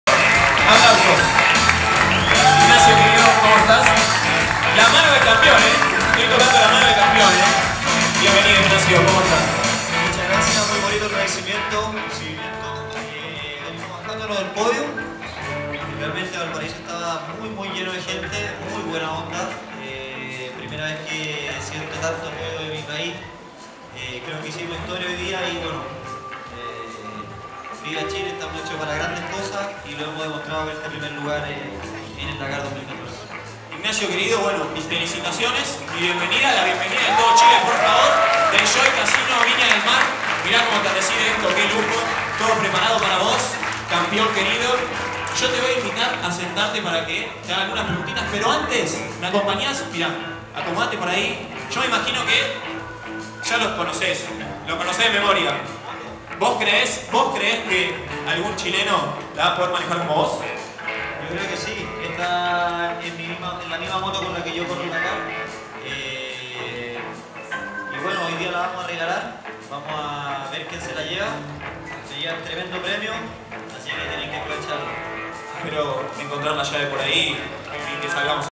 Dakar Ignacio Casale sorteo en Casino Enjoy
Dakar presentación de Ignacio Casale en Casino Enjoy Viña
Team Enjoy Viña (Host Esteban)
Productora: Silvana Pane